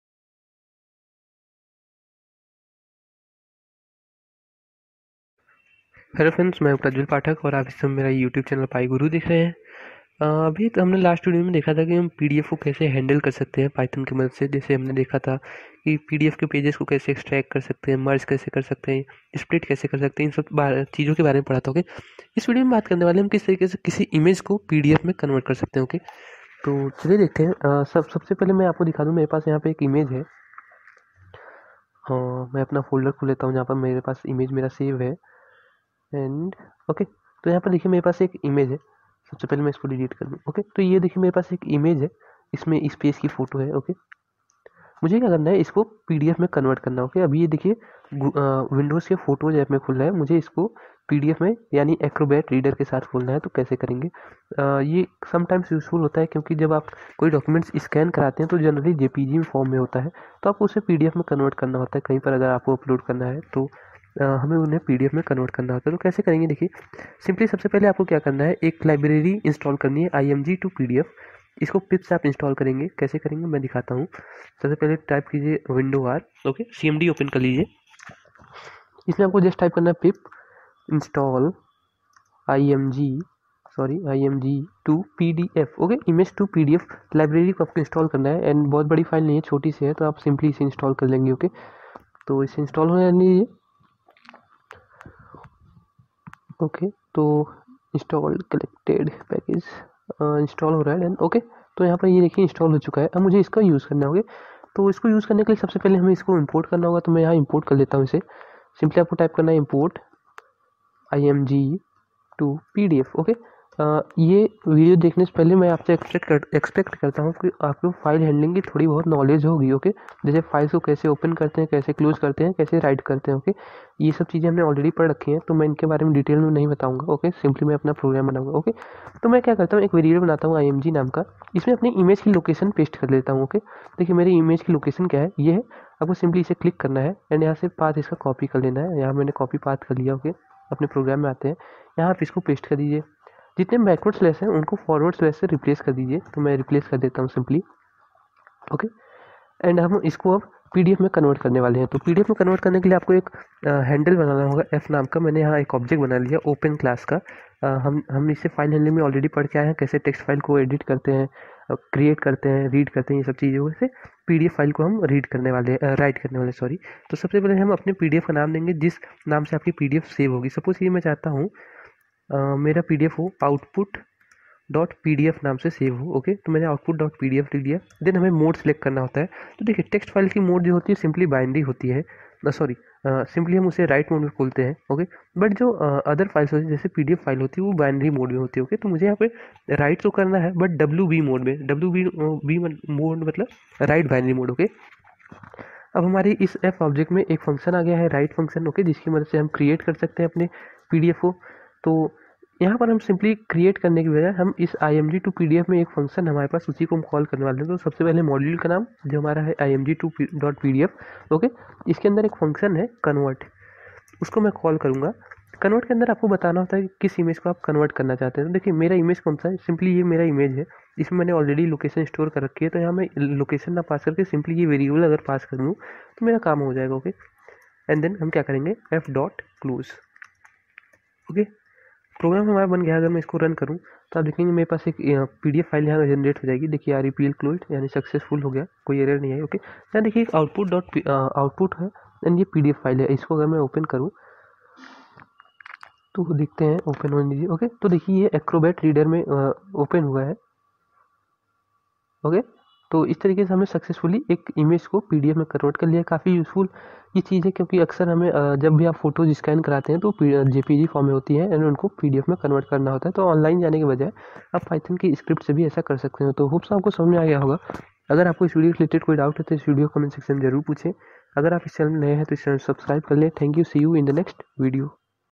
pyPDF 10 | Converting images to pdf | pyGuru
Hello YouTube,
      In this video we'll be talking about how can we convert a image to pdf using python